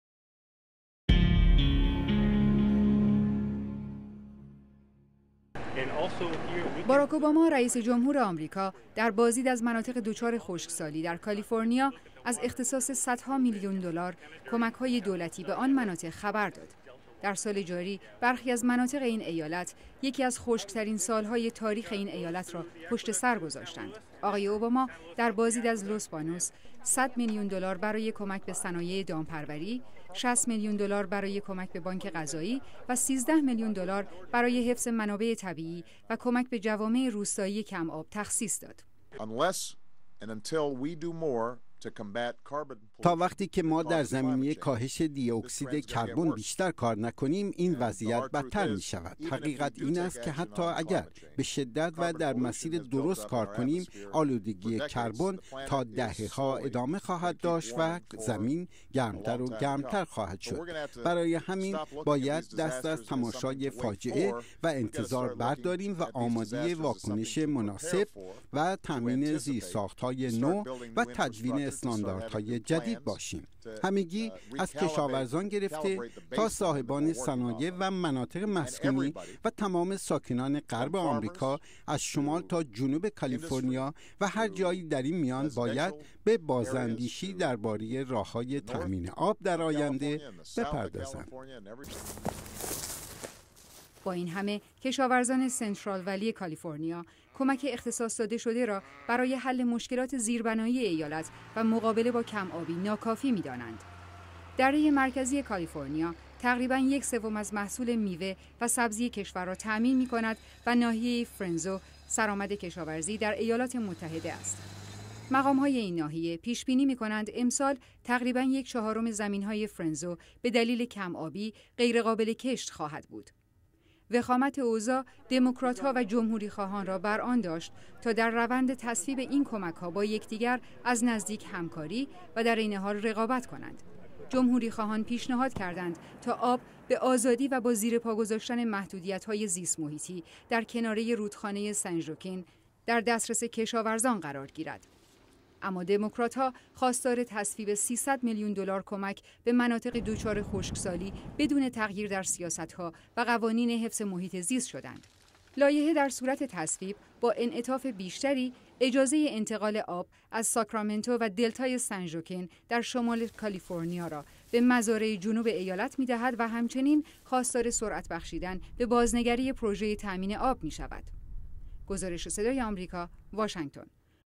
نیاز به بازنگری در برنامه های زیست محیطی کاليفرنيا
باراک اوباما، رییس جمهور آمریکا، ضمن دیدار از مناطق دچار خشکسالی در کالیفرنیا، درباره خطر تغییرات اقلیمی و نیاز به بازنگری در برنامه های زیست محیطی این ایالت سخن گفت.